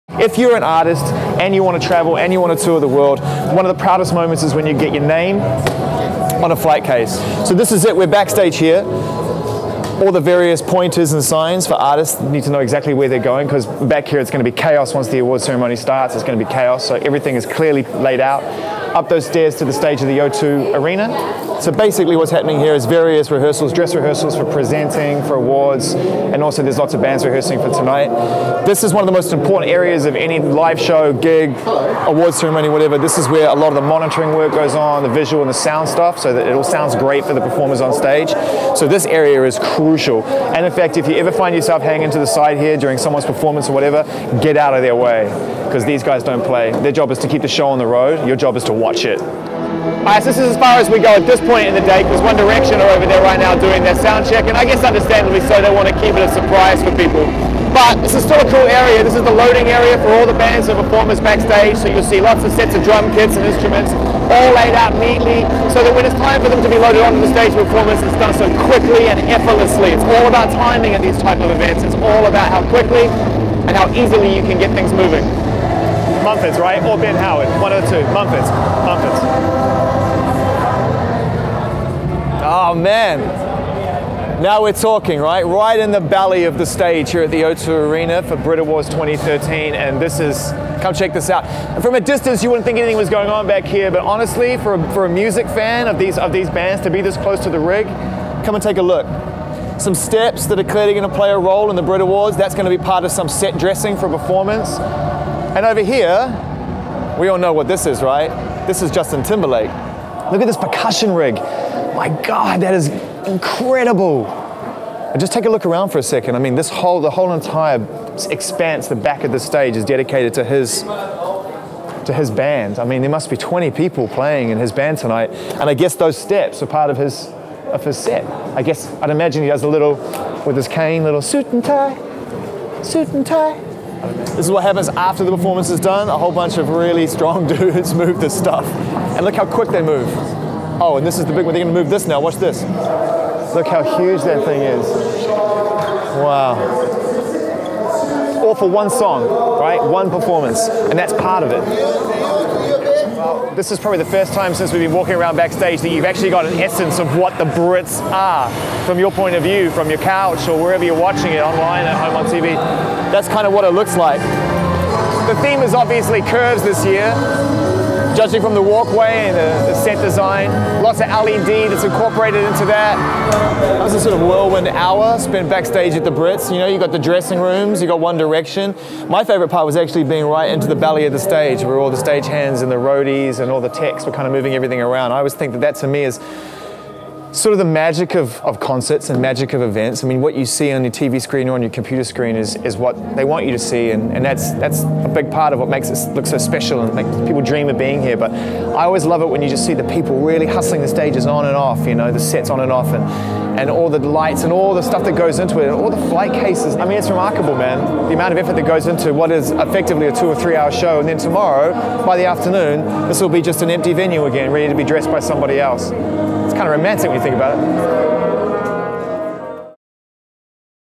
Zane Lowe Tours the 2013 BRIT Awards Stage
Ever wonder what goes on behind the stage? Zane Lowe shows you the ropes.